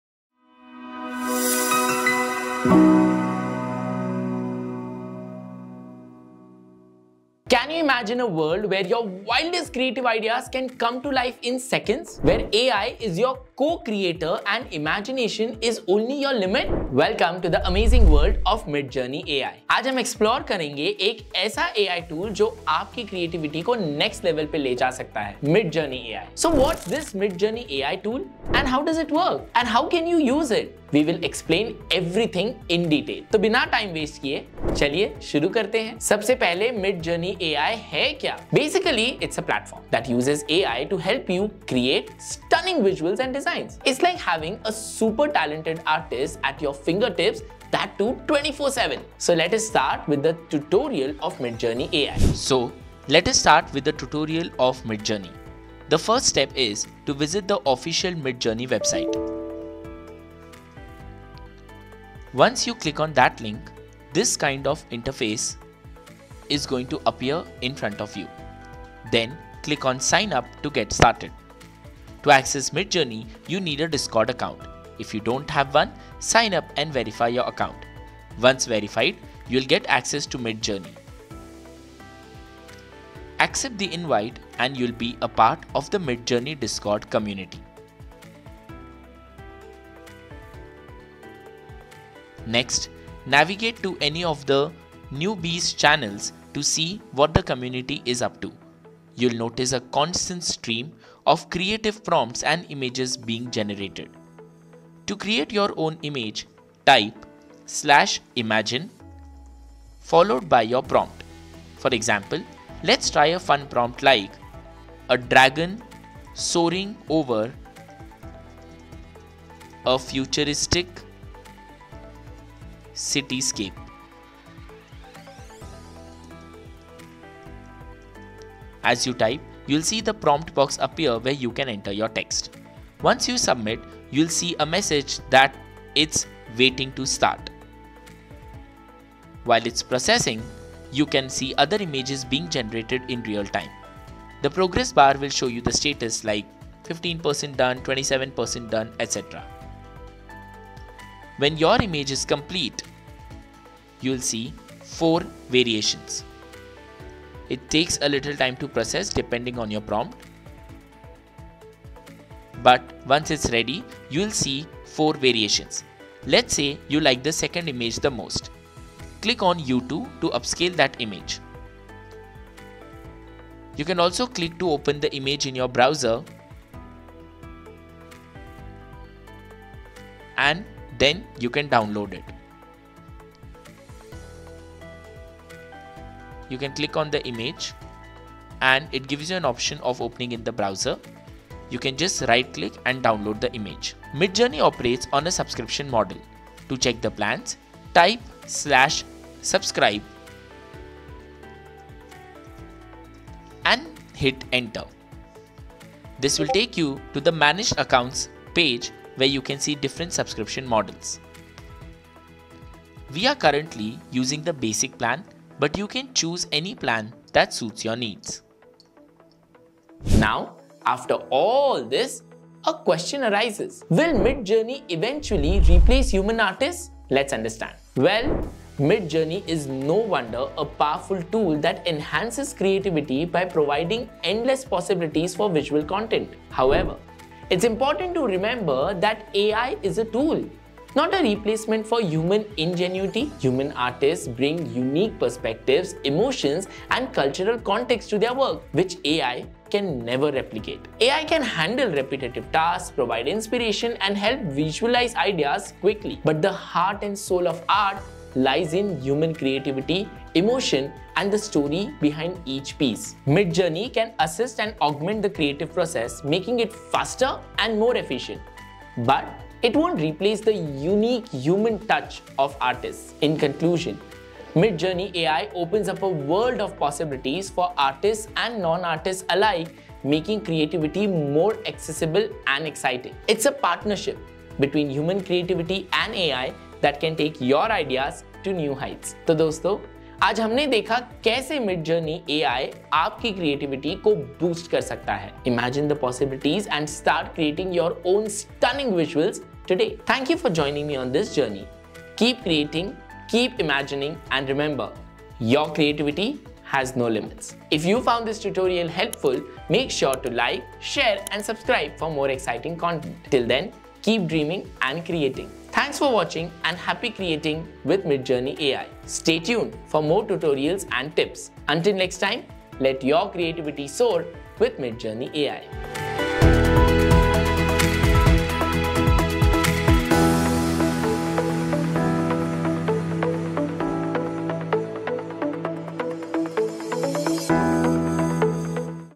Learn How To Use Midjourney AI Under 5 Minutes | Midjourney AI Tutorial | Learnbay
In this quick and easy tutorial, Learnbay shows you how to get started with Midjourney AI and unlock its full potential. Learn the basics, explore its features, and discover how to use Midjourney AI to enhance your creativity and productivity.

Perfect for beginners, this tutorial covers:

✨ Setting up Midjourney AI
✨ Understanding the interface
✨ Generating images and ideas
✨ Tips and tricks for getting the most out of Midjourney AI